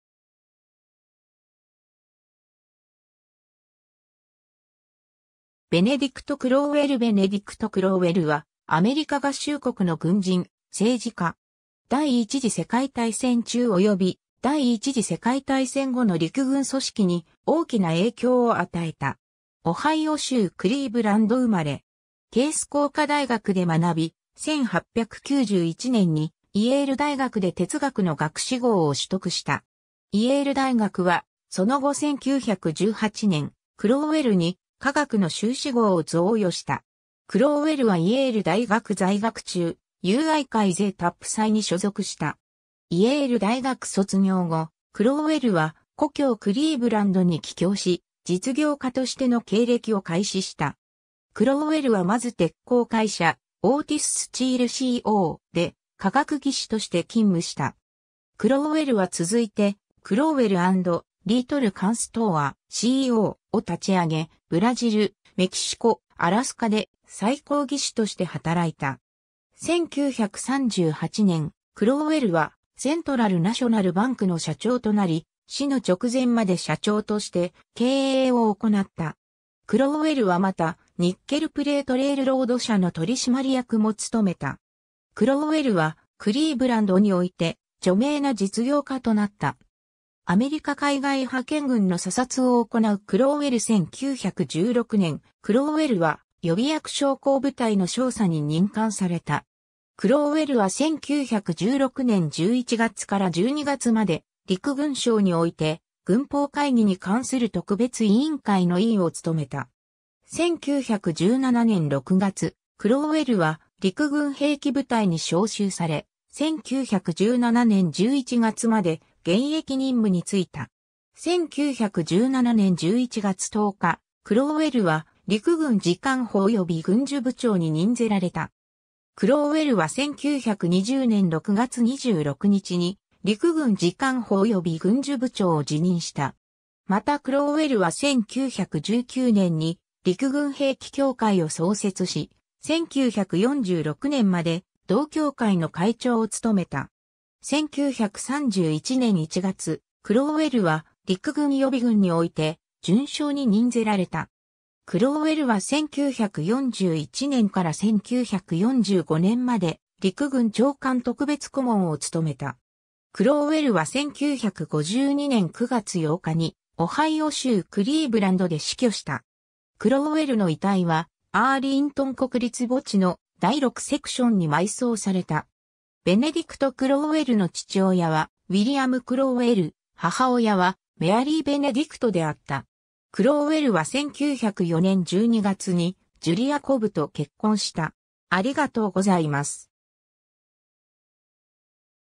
ベネディクト・クロウウェル
ベネディクト・クロウウェル ベネディクト・クロウウェル（Benedict Crowell, 1869年10月21日 - 1952年9月8日）は、アメリカ合衆国の軍人、政治家。
第一次世界大戦中および第一次世界大戦後の陸軍組織に大きな影響を与えた。
 オハイオ州クリーブランド生まれ。
ケース工科大学で学び、1891年にイェール大学で哲学の学士号を取得した。
イェール大学はその後1918年、クロウウェルに化学の修士号を贈与した。
クロウウェルはイェール大学在学中、友愛会ゼータ・プサイに所属した。
 イェール大学卒業後、クロウウェルは故郷クリーブランドに帰郷し、実業家としての経歴を開始した。
クロウウェルはまず鉄鋼会社 Otis Steel Co. で化学技師として勤務した。
クロウウェルは続いて Crowell & Little Constr. Co. を立ち上げ、ブラジル、メキシコ、アラスカで採鉱技師として働いた。
1938年、クロウウェルは Central National Bank の社長となり、死の直前まで社長として経営を行った。
クロウウェルはまた、Nickel Plate Railroad 社の取締役も務めた。
クロウウェルはクリーブランドにおいて、著名な実業家となった 。
 アメリカ海外派遣軍の査察を行うクロウウェル 1916年、クロウウェルは予備役将校部隊の少佐に任官された。
クロウウェルは1916年11月から12月まで、陸軍省において軍法会議に関する特別委員会の委員を務めた。
1917年6月、クロウウェルは陸軍兵器部隊に召集され、1917年11月まで現役任務に就いた。
1917年11月10日、クロウウェルは陸軍次官補および軍需部長に任ぜられた。
クロウウェルは1920年6月26日に陸軍次官補および軍需部長を辞任した。
またクロウウェルは1919年に陸軍兵器協会を創設し、1946年まで同協会の会長を務めた。
 1931年1月、クロウウェルは陸軍予備軍において、准将に任ぜられた。
クロウウェルは1941年から1945年まで、陸軍長官特別顧問を務めた。
 クロウウェルは1952年9月8日にオハイオ州クリーブランドで死去した。
クロウウェルの遺体はアーリントン国立墓地の第6セクションに埋葬された。
 ベネディクト・クロウウェルの父親はウィリアム・クロウウェル (William Crowell)、母親はメアリー・ベネディクト (Mary Benedict) であった。
クロウウェルは1904年12月にジュリア・コブ (Julia Cobb) と結婚した。